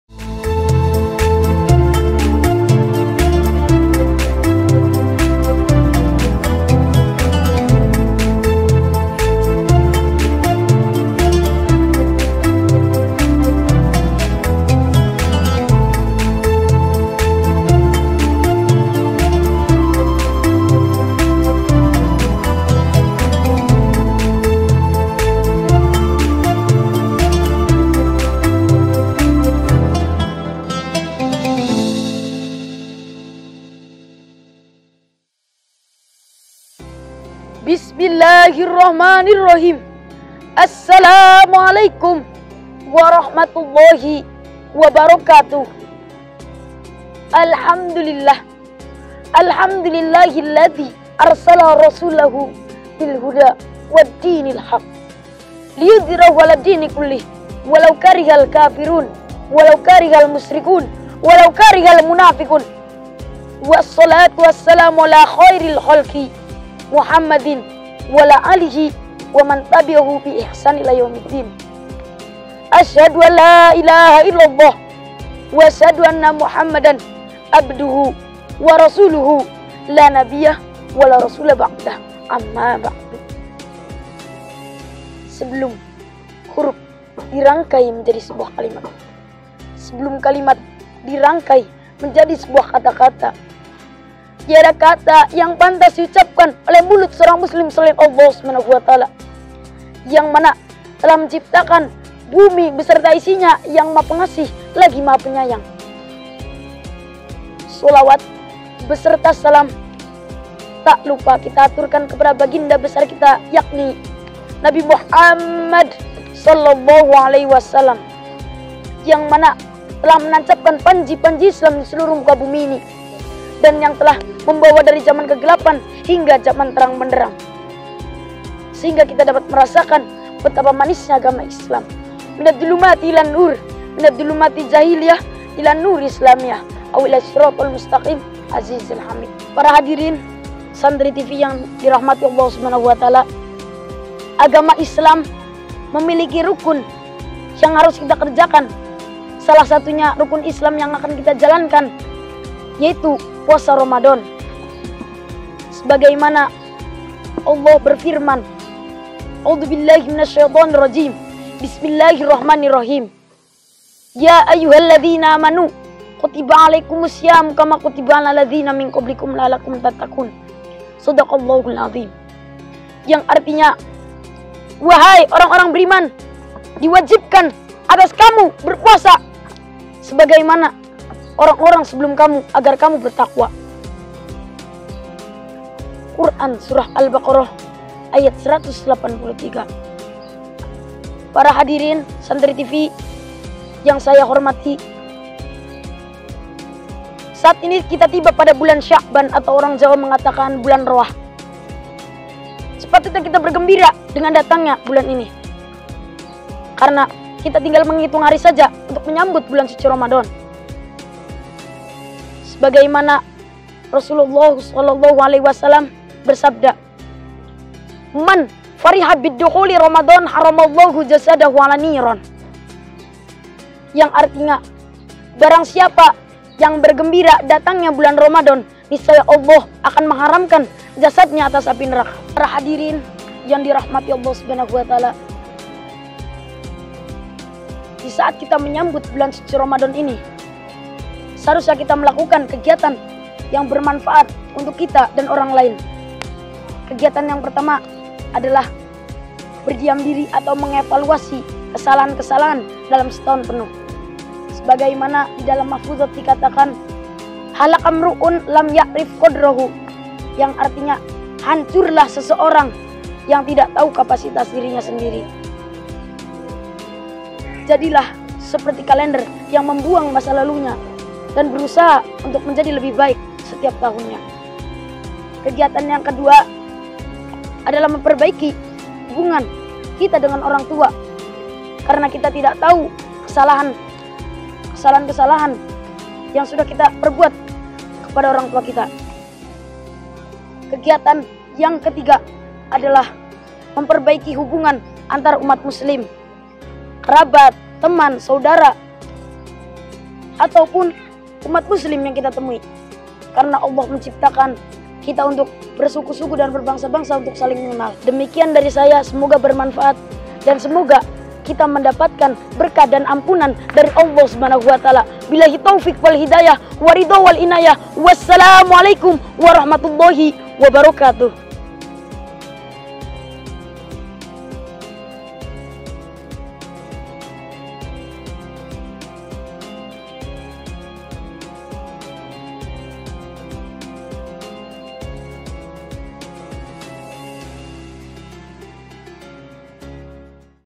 3 Persiapan Sambut Ramadhan | Kajian Indah Ramadhan | Darunnajah 14
Sahabat yang dimuliakan Allah SWT, anak-anak santri dan alumni yang kami banggakan, jangan lupa ya stay tune di Youtube, FB n Instagram Darunnajah 14 dalam KIAM (Kajian Indah Ramadhan) bersama:

Rafi Bahaudin (Santri Kelas 1 TMI)

Persembahan dari @SantriTV

SERIES KAJIAN INDAH RAMADHAN (KIAM)

Episode 1 ini akan bersama belajar tentang:

3 Persiapan Sambut Ramadhan